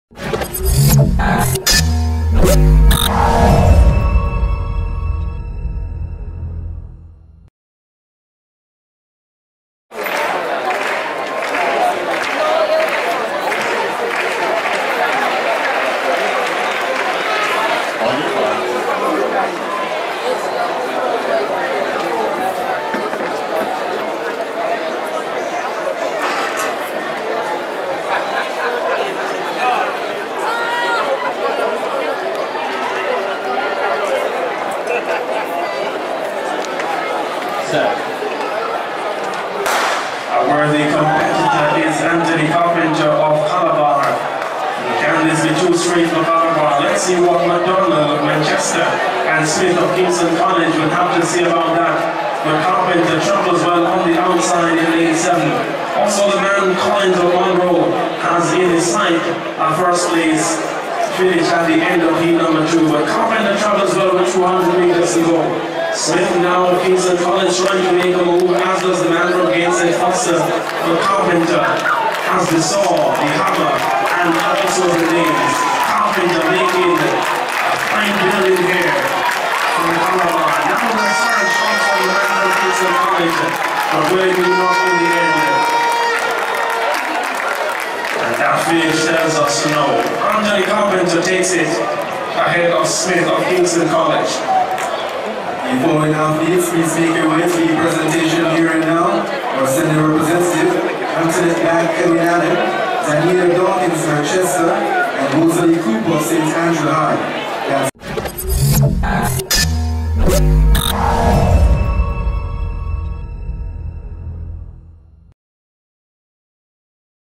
C'Bar's Anthony Carpenter wins Boys Class 2 400m - Youngster Goldsmith - ROAD TO CHAMPS 2014
WATCH as the promising Anthony Carpenter wins his HEAT of the Boys Class 2 400 metres easily in 49.63 seconds at the Youngster Goldsmith Classics inside the National Stadium in Kingston, Jamaica on February 1, 2014. The Calabar High School standout was the OVERALL CHAMPION finishing with the FASTEST TIME in all heats (88 competitors).

(VIDEO RECORDED by: SportsXplorer Multimedia) in Kingston, Jamaica.

RESULT - TOP 20 FROM ALL HEATS (88 competitors)
Place// Name// Age// School// Time//
1 Carpenter, Anthony LYONS (Calabar High School) 49.63
2 francis, akyeeme Calabar High Sch 49.67
3 Ellignton, Devaughn 15 Petersfield High 50.27
4 Heath, Carlo 15 Manchester High 50.47
5 porter, gawayne Calabar High Sch 50.56
6 Walker, Jelani Bluestreak 50.57
7 Noble, Denroy Calabar High Sch 50.83
8 Barrett, Timar 16 Monk Street Boys 50.93
9 Ledgister, Leonardo 14 St. Elizabeth TE 51.13
10 Stewart, Joel 16 Monk Street Boys 51.18
11 Smith, Jean Marc Kingston College 51.24
12 James, Jauavney 15 St. Elizabeth TE 51.32
13 Barnes, Shamar 15 Monk Street Boys 51.45
14 Morris, Dashawn Kingston College 51.55
15 Fearon, Raje Kingston College 51.94
16 Thompson, Achebe 14 Excelsior High 51.97
17 Hyde, Craig Bluestreak 52.02
18 Lenmonious, Phillip Jamaica College 52.10
19 Francis, Nicholas 15 Camperdown High 52.29
20 Campbell, Ayoka 15 Excelsior High 52.32